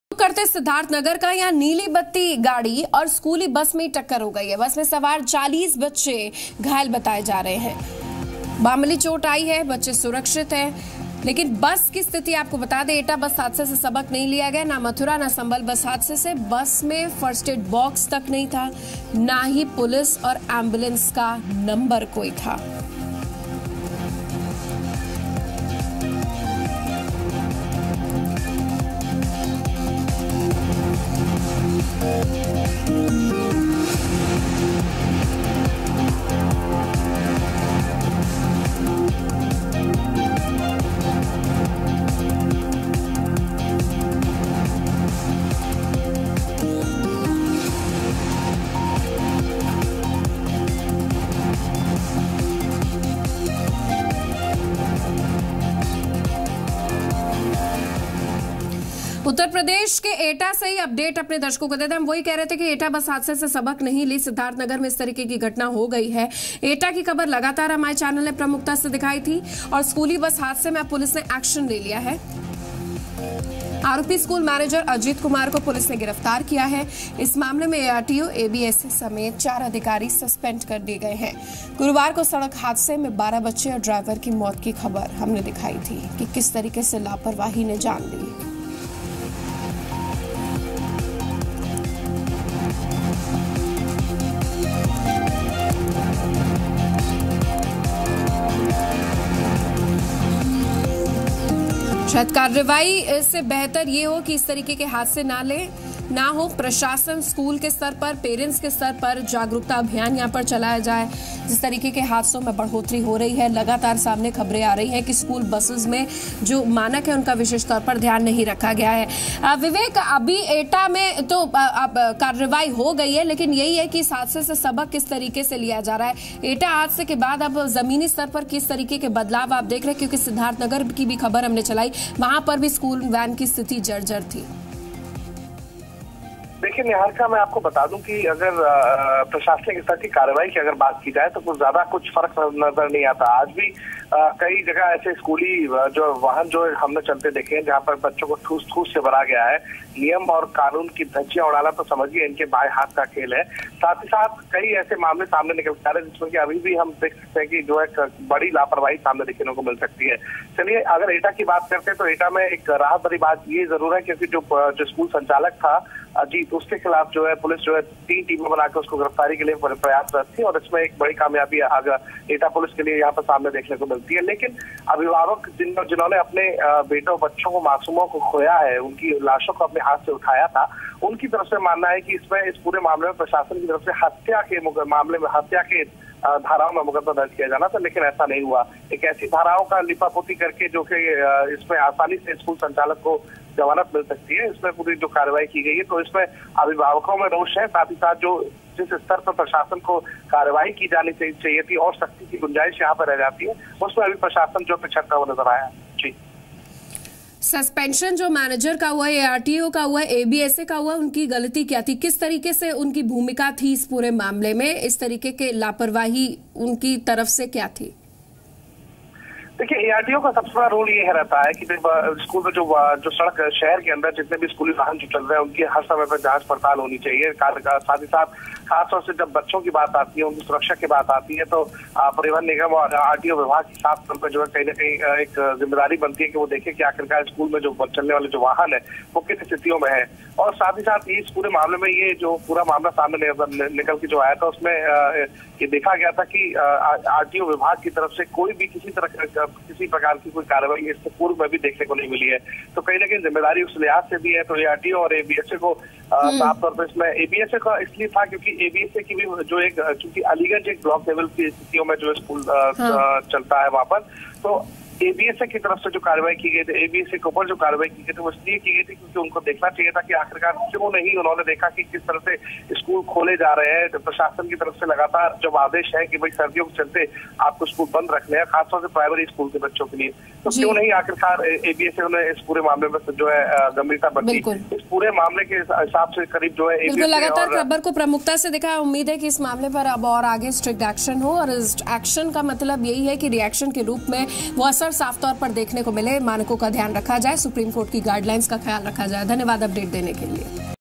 Siddharthnagar : Head-on collission of a school bus and a car, students safe with minor injuries
Head-on collission between a school bus and a car, students safe with minor injuries. Watch complete news story of Badi Khabar to get the detailed news updates!